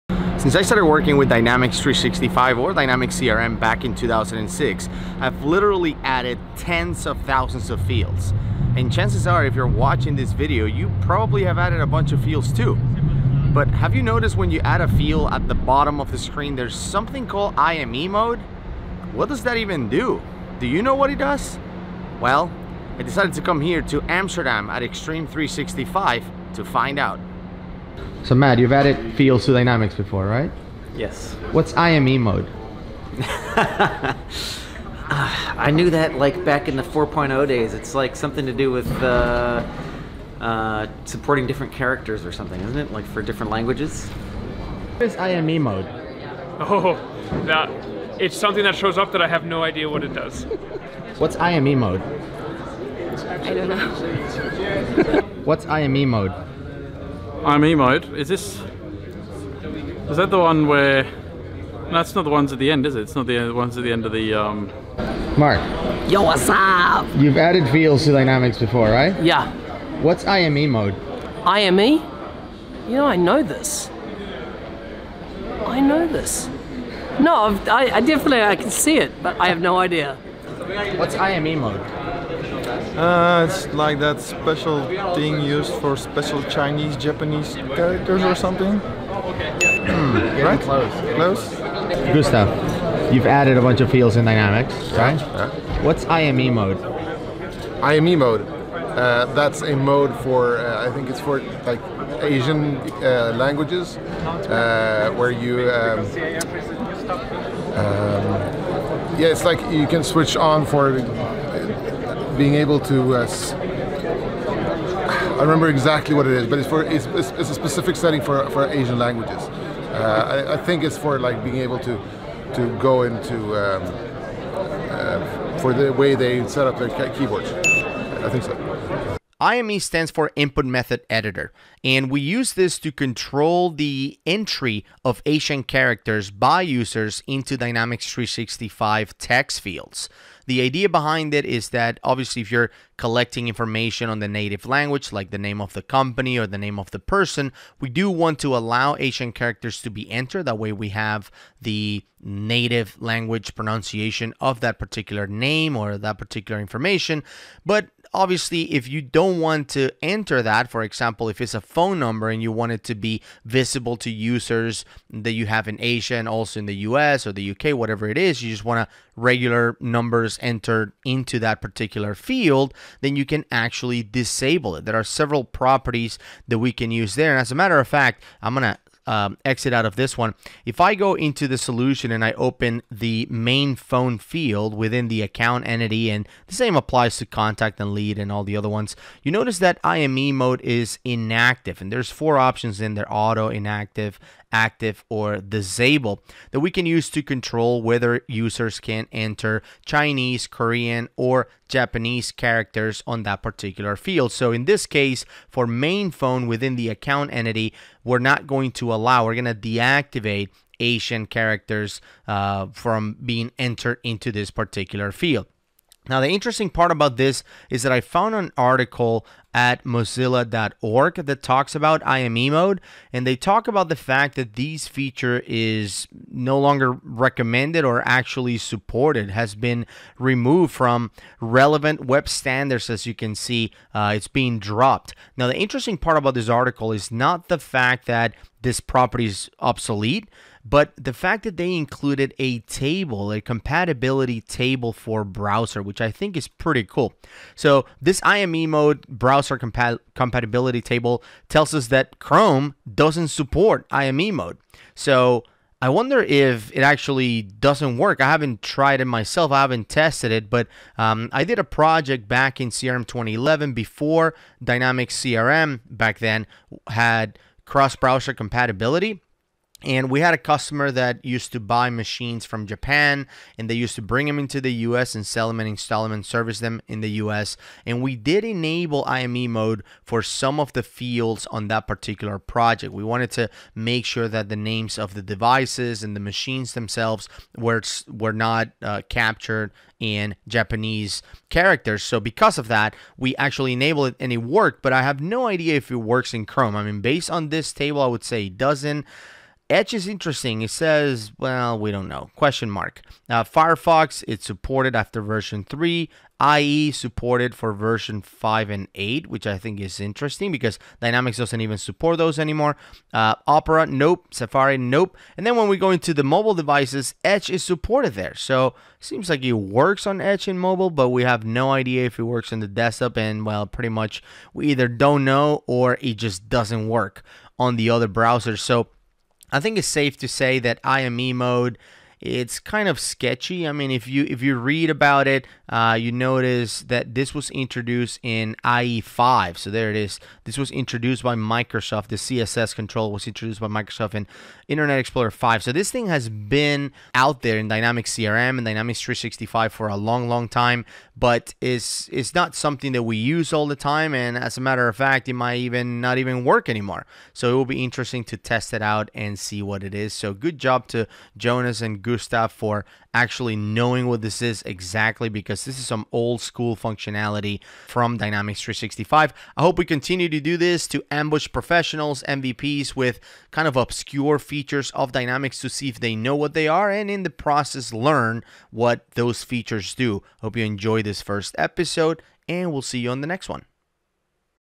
Do you know Dynamics 365? - Episode 1: What's IME Mode?
I ambush several Dynamics 365 experts, including several Microsoft MVPs and ask them "What's IME Mode?" - a feature that has been in Dynamics CRM/365 for over 10 years - to see if they know.